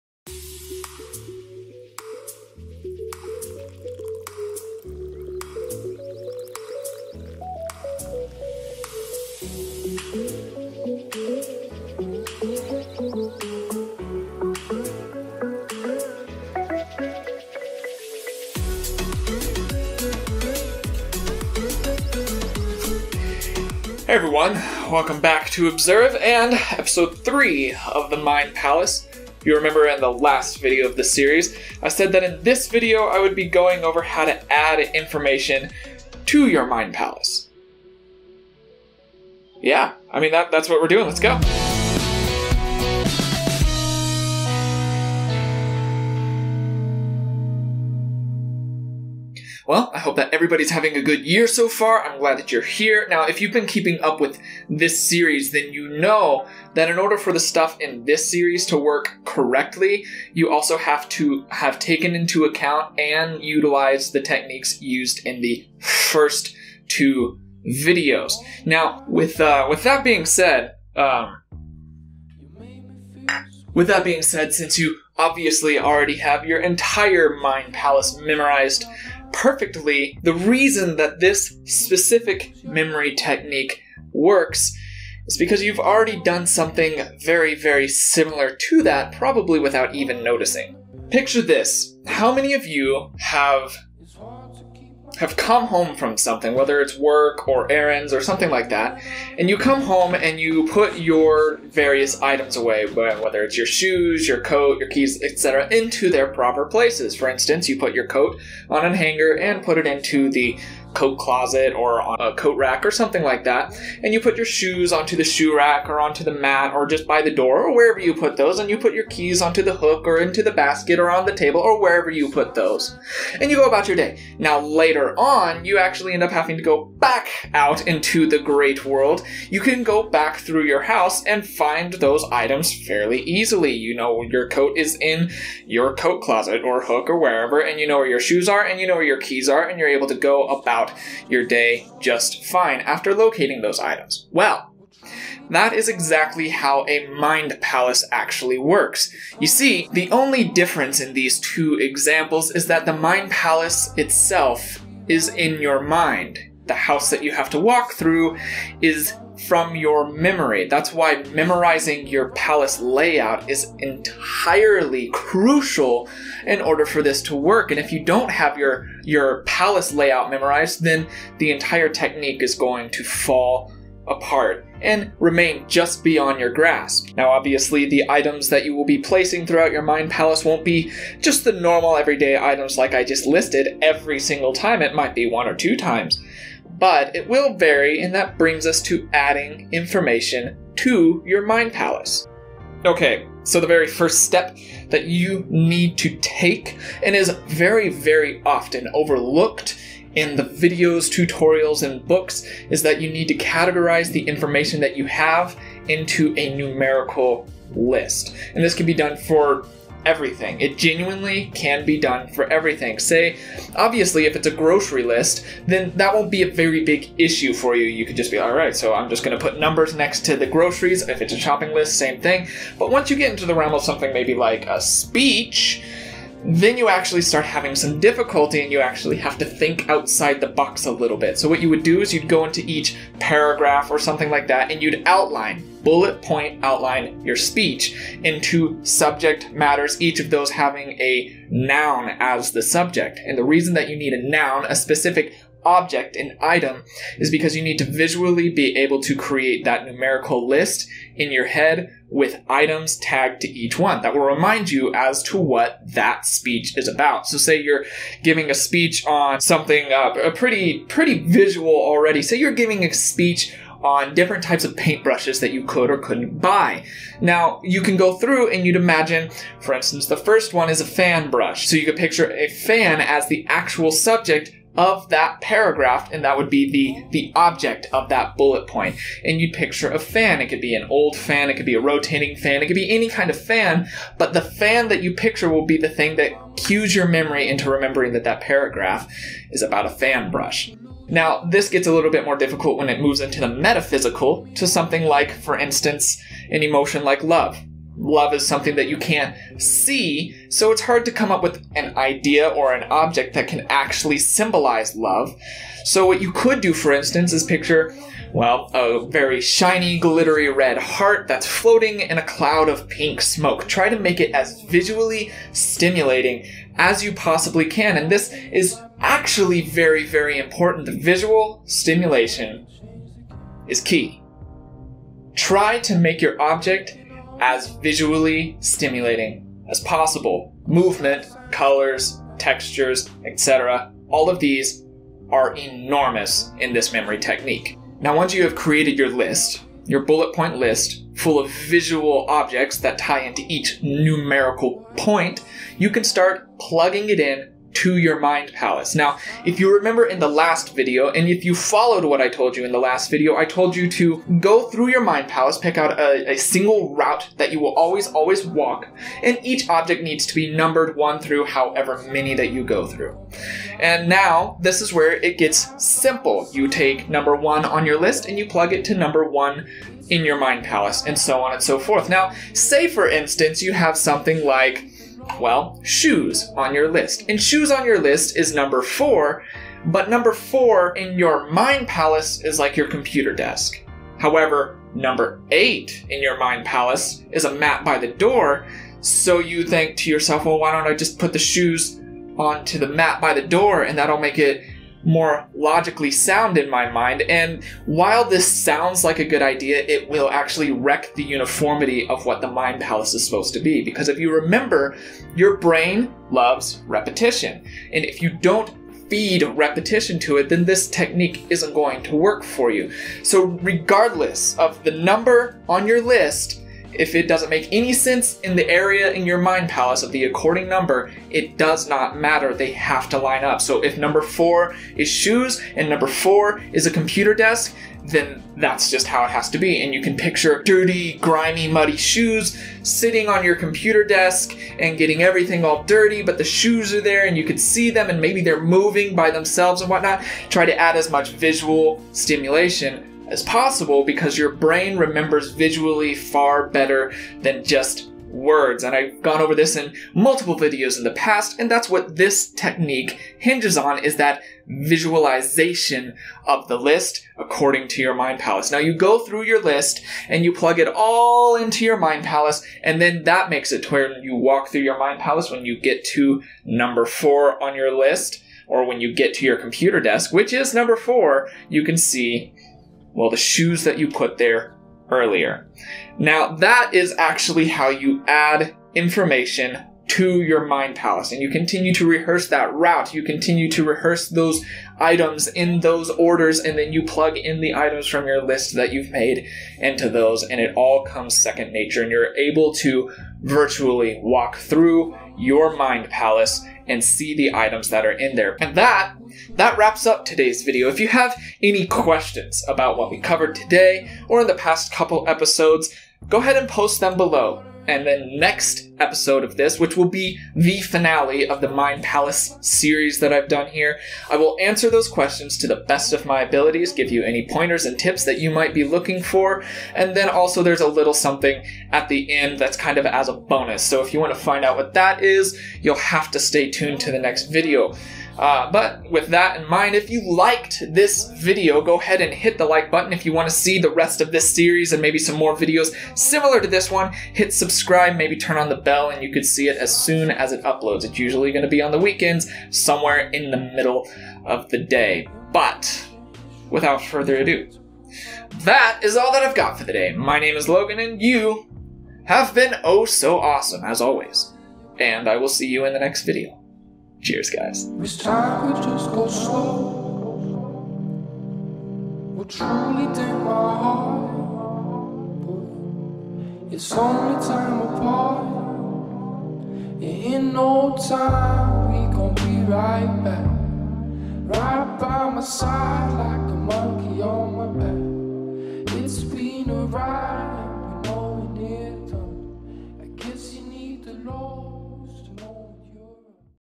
HOW TO USE YOUR MIND PALACE | Mind Palace: Adding Information
Here's the third installment of this series of the Mind Palace. This is how you add information to your palace. I hope you find this useful! Cheers guys.